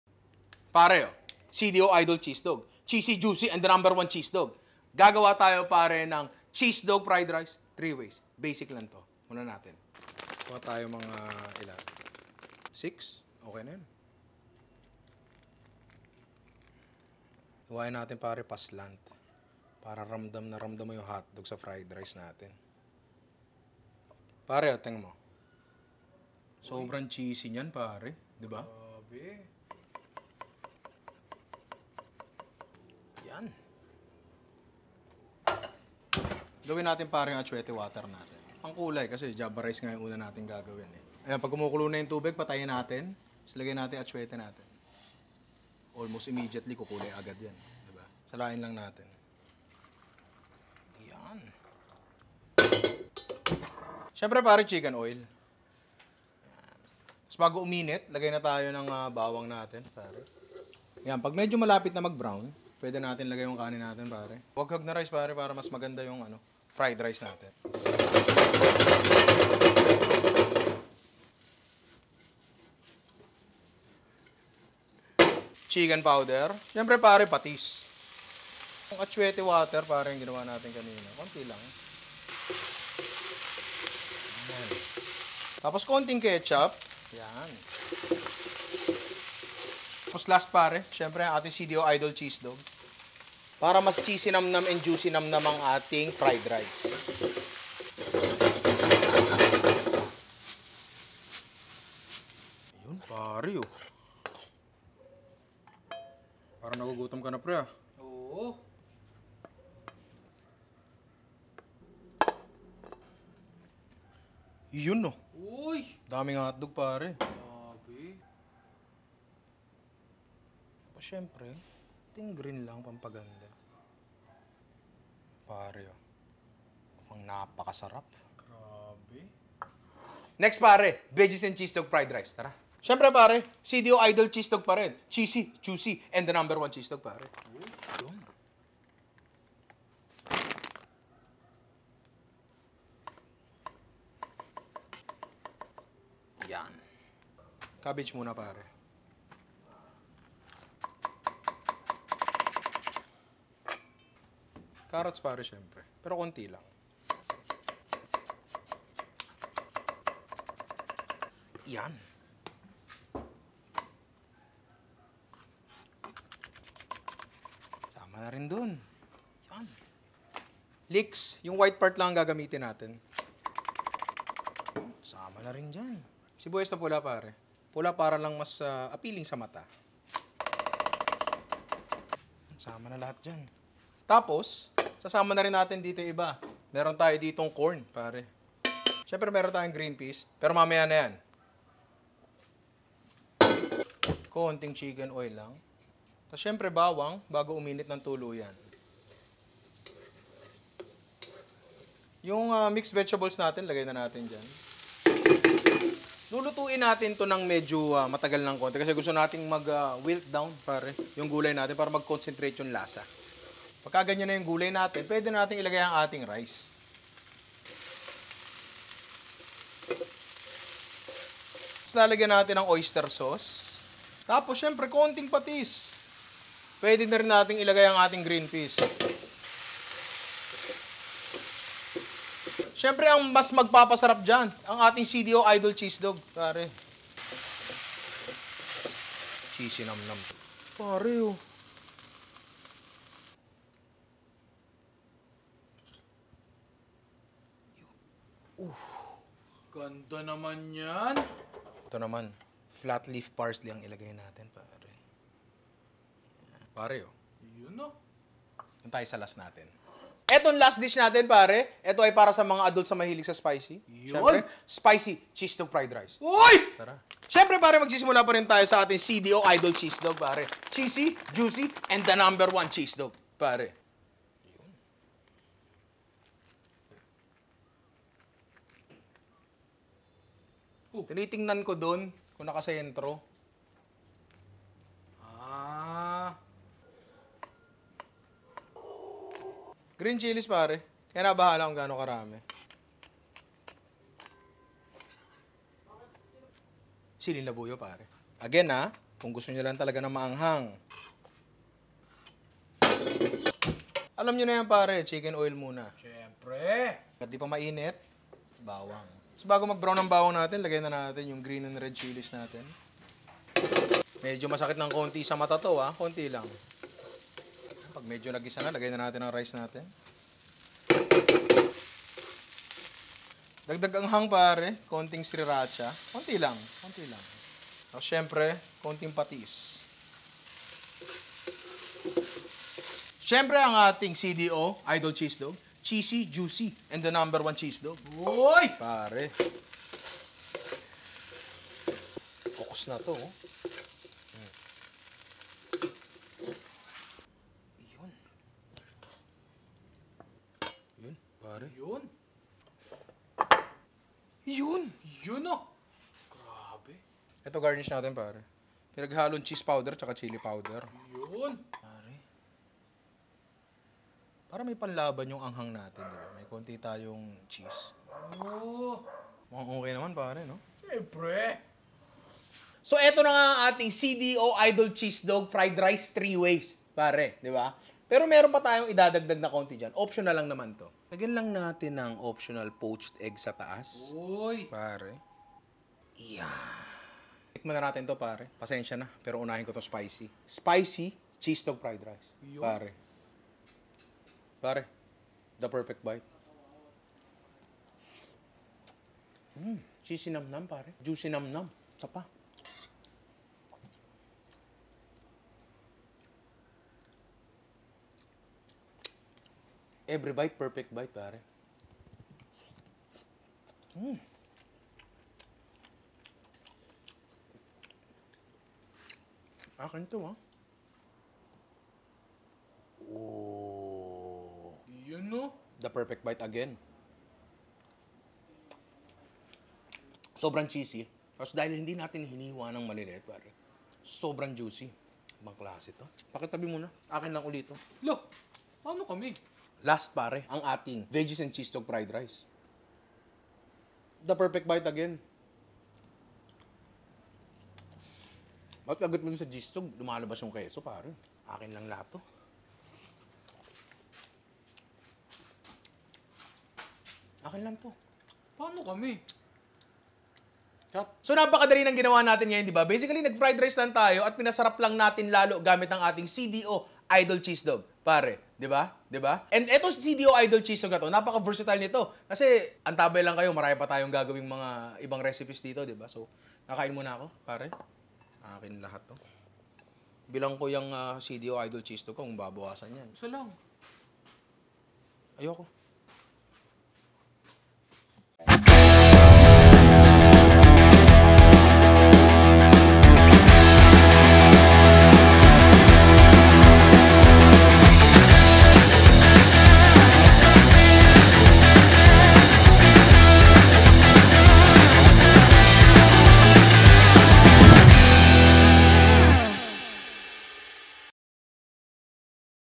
CHEESEDOG FRIED RICE 3 WAYS | Ninong Ry
CheesyNamnam at JuicyNamnam!

Ang CDO Idol Cheesedog ay available sa mga supermarkets and online stores via official CDO Lazada store. Bili na kayo mga inaanak!